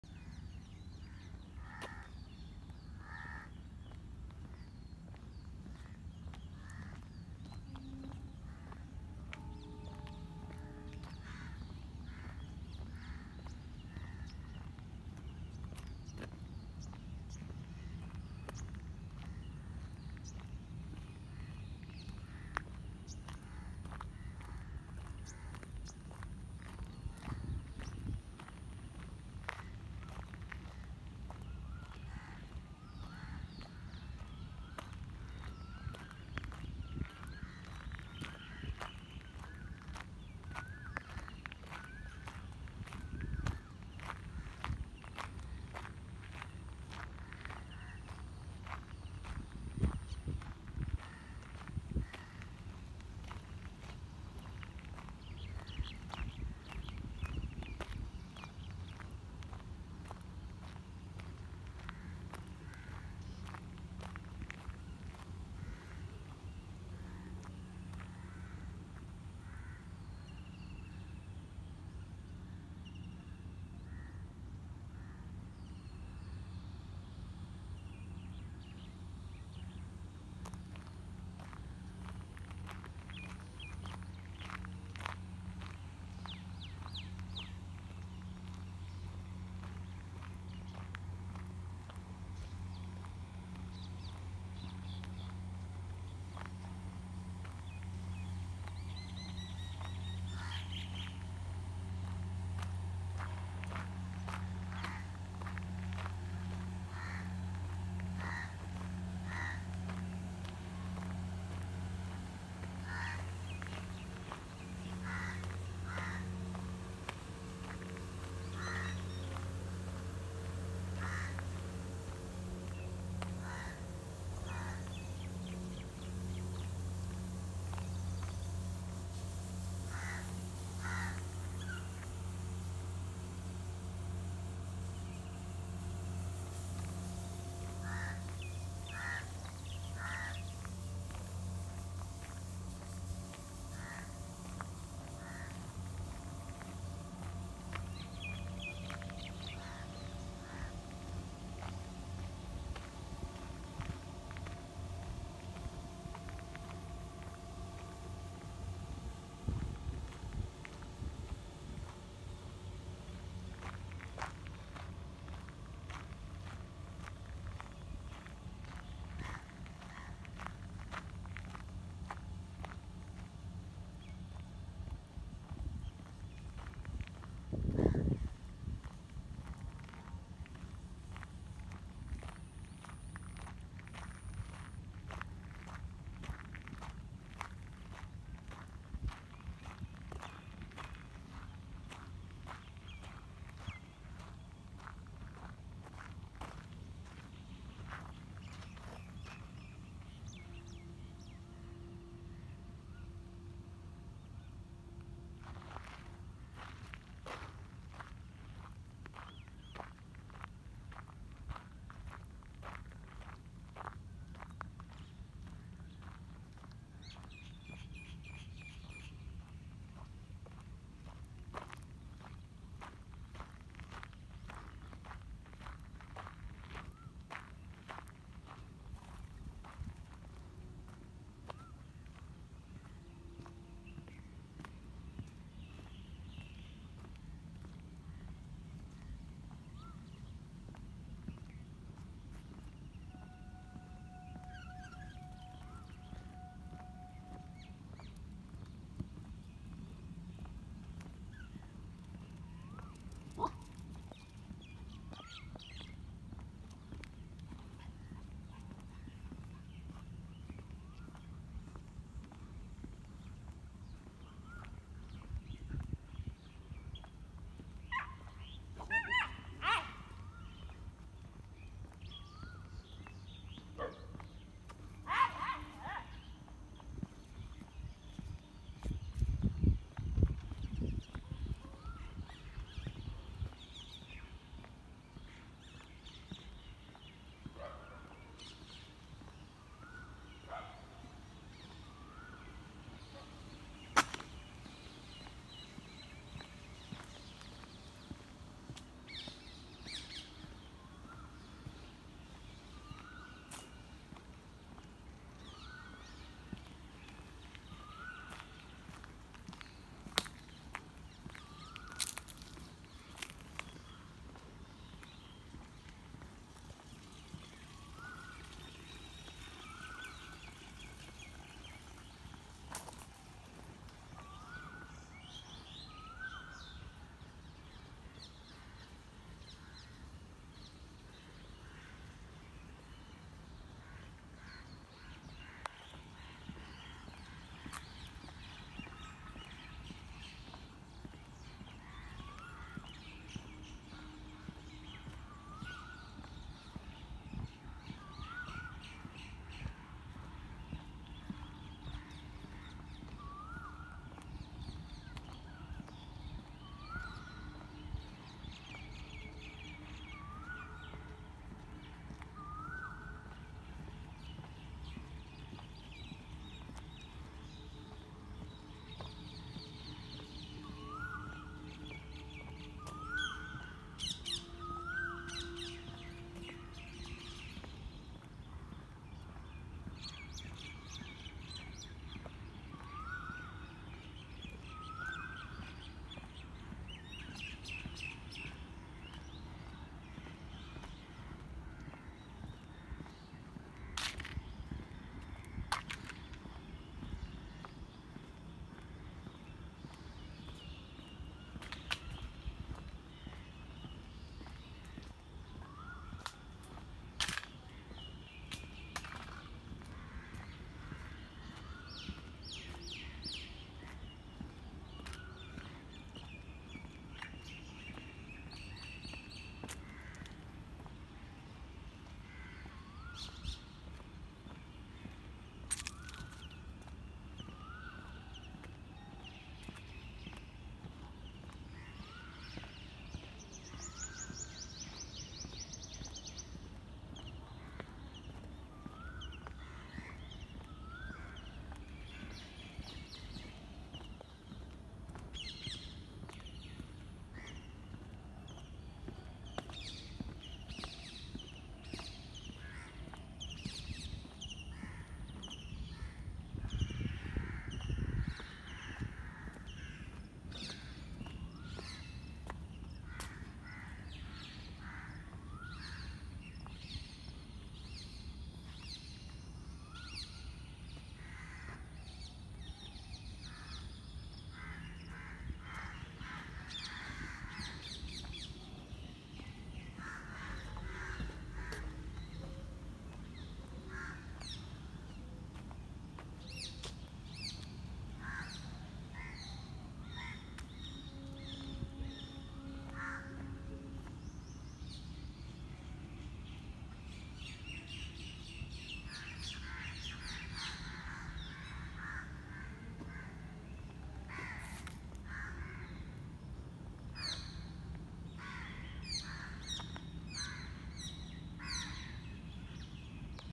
Going around VS Hall , NIT,Rourkela. Second largest hall in Asia?
We walk around Vikram Sarabhai Hall of Residence in NIT,Rourkela to see how it looks like from outside.
Vikram Sarabhai Hall of Residence, reputed to be the second largest hall in Asia, houses around 1300 students.